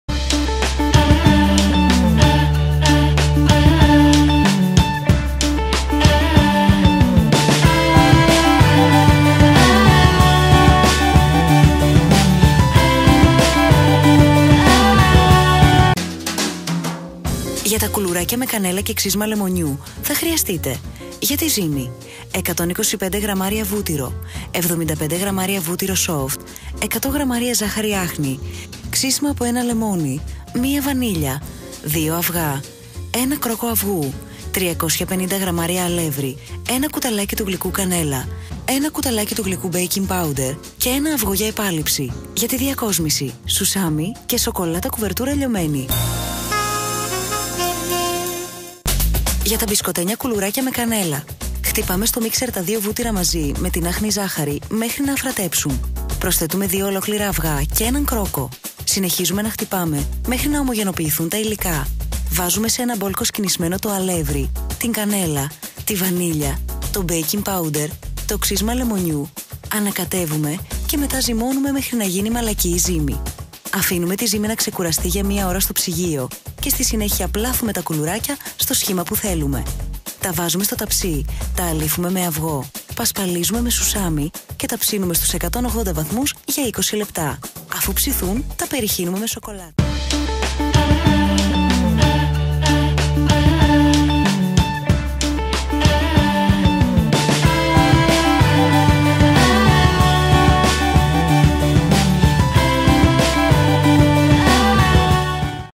Κουλουράκια με κανέλλα & ξύσμα λεμονιού
ΥΛΙΚΑ

Για τη ζύμη:
125 γρ. βούτυρο Lurpak (σε θερμοκρασία δωματίου)
75 γρ. βούτυρο soft Lurpak (σε θερμοκρασία δωματίου)
100 γρ. ζάχαρη άχνη
Ξύσμα από 1 λεμόνι
1 βανίλια
2 αβγά
1 κρόκο αβγού
350 γρ. αλεύρι Αλλατίνη
1 κ.γ. κανέλλα
1 κ.γ. μπέικιν πάουντερ Αλλατίνη
1 αβγό (για επάλειψη)
Για τη διακόσμηση:
Σουσάμι
Σοκολάτα κουβερτούρα λιωμένη

ΕΚΤΕΛΕΣΗ

1. Χτυπάμε στο μίξερ, με το φτερό, τα δύο βούτυρα μαζί με την άχνη ζάχαρη, μέχρι να αφρατέψουν.
2. Χτυπάμε ελαφρώς 2 αβγά, τα προσθέτουμε στο μείγμα μαζί με τον κρόκο και συνεχίζουμε να χτυπάμε μέχρι να ομογενοποιηθούν τα υλικά.
3. Βάζουμε σε ένα μπολ κοσκινισμένο το αλεύρι, την κανέλλα, τη βανίλια, το μπέικιν πάουντερ, το ξύσμα λεμονιού και ανακατεύουμε.
4. Προσθέτουμε το μείγμα με το αλεύρι στον κάδο του μίξερ και ζυμώνουμε με το χέρι, μέχρι να γίνει μαλακή η ζύμη.
5. Αφήνουμε τη ζύμη να ξεκουραστεί για μία ώρα στο ψυγείο και στη συνέχεια πλάθουμε τα κουλουράκια στο σχήμα που θέλουμε.
6. Τα βάζουμε σε ένα ταψί αντικολλητικό ή με λαδόκολλα, αλείφουμε την επιφάνειά τους με αβγό και τα πασπαλίζουμε με σουσάμι.
7. Έπειτα τα ψήνουμε, σε προθερμασμένο φούρνο, στους 180 βαθμούς, για 20 λεπτά.
8. Αφού ψηθούν περιχύνουμε τα μπισκότα με λιωμένη σοκολάτα.

Από την μαγείρισσα Τασούλα Κουφοπούλου

Κάθε μέρα μαζί σας με μια καινούργια Συνταγή!